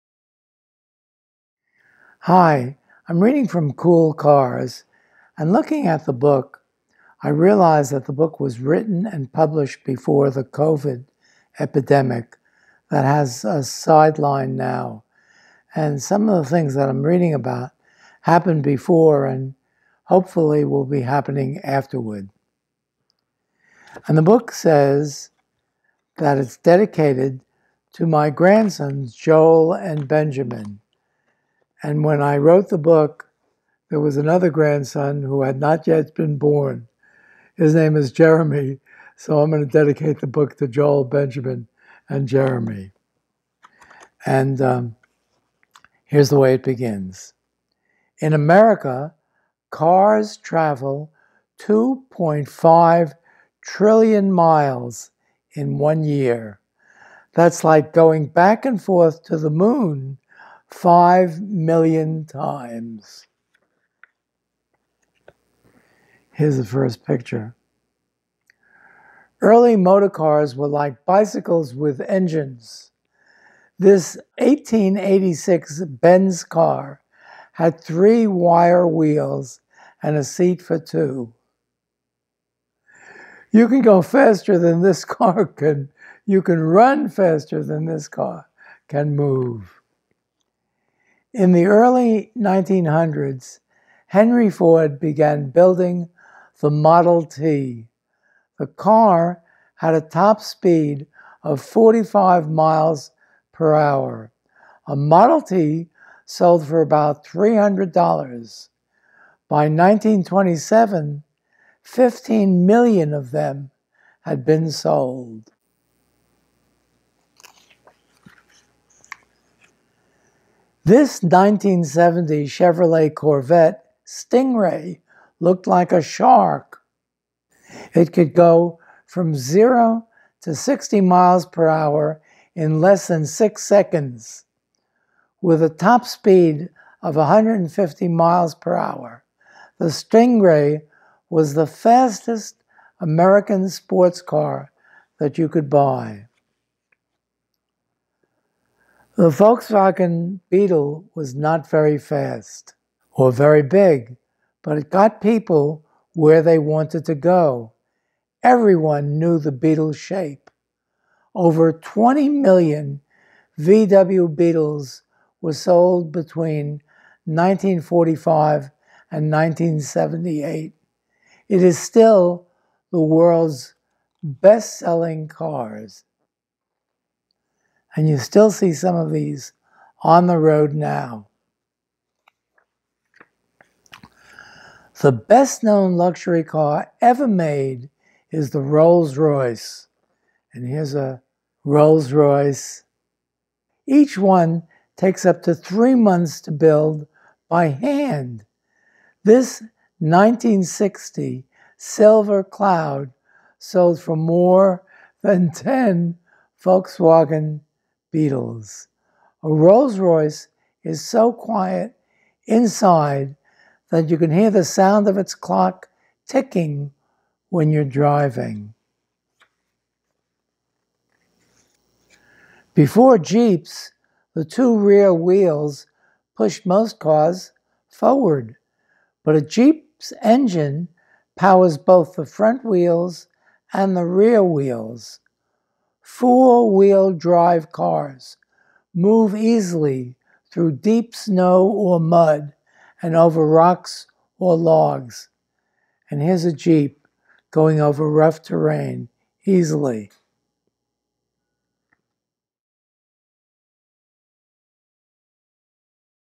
COOL CARS (Beginning) | Seymour Simon Reads Aloud
American cars travel two-and-a-half trillion miles in a year - that's five million round trips to the moon! Learn all about cars --- classics, race cars, and vehicles of the future --- as renowned science author Seymour Simon reads aloud from one of his most popular books, COOL CARS (StarWalk Kids Media, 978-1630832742).